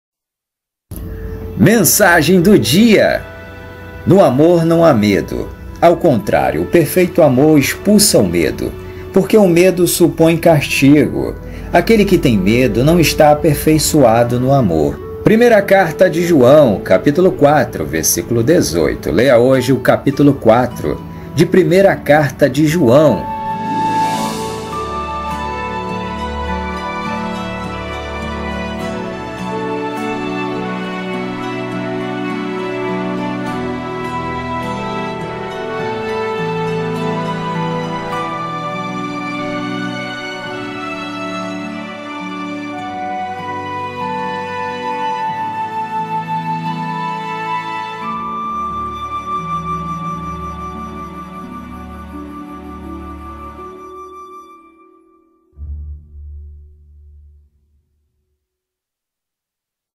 Mensagem do Dia! 1 João 4:18
No amor não há medo; ao contrário o perfeito amor expulsa o medo, porque o medo supõe castigo. Aquele que tem medo não está aperfeiçoado no amor.
1 João 4:18